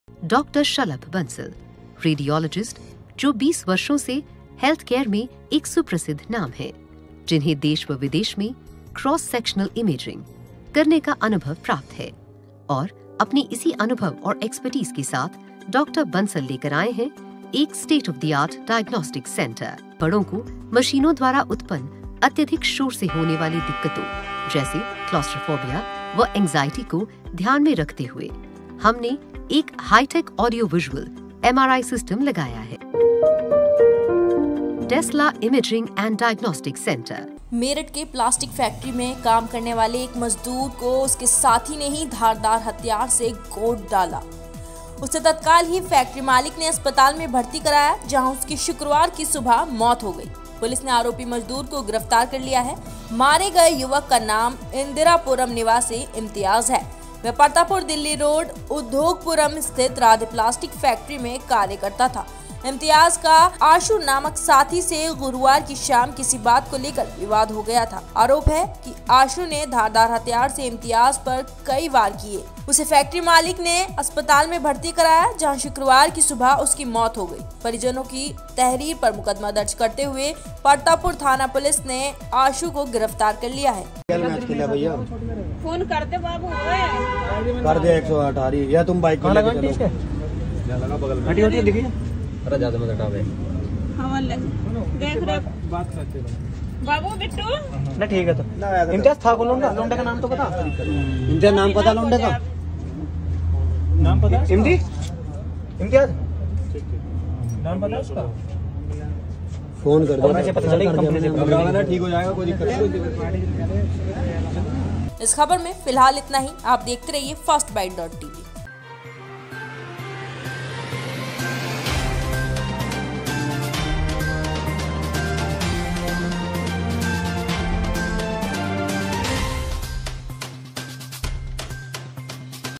मेरठ में प्लास्टिक फैक्ट्री में हत्या, आरोपी गिरफ्तार - First Byte Tv
मेरठ में प्लास्टिक फैक्ट्री में काम करने वाले एक मजदूर को उसके साथी ने ही धारदार हथियार से गोद डाला।  उसे तत्काल ही फैक्ट्री मालिक ने अस्पताल में भर्ती कराया जहां उसकी शुक्रवार की सुबह मौत हो गयी। पुलिस ने आरोपी मजदूर को गिरफ्तार कर लिया है । मारे गये युवक का नाम इंदिरापुरम निवासी इम्तियाज है। वह परतापुर दिल्ली रोड उद्योगपुरम स्थित राधे प्लास्टिक फैक्ट्री में कार्य करता था। इम्तियाज का आशु नामक साथी से गुरूवार की शाम किसी बात को लेकर विवाद हो गया था। आरोप है कि आशु ने धारदार हथियार से इम्तियाज पर कई वार कर दिये। उसे फैक्ट्री मालिक ने अस्पताल में भर्ती कराया जहां शुक्रवार की सुबह उसकी मौत हो गयी। परिजनों की तहरीर पर मुकदमा दर्ज करते हुए परतापुर थाना पुलिस ने आशु को गिरफ्तार कर लिया है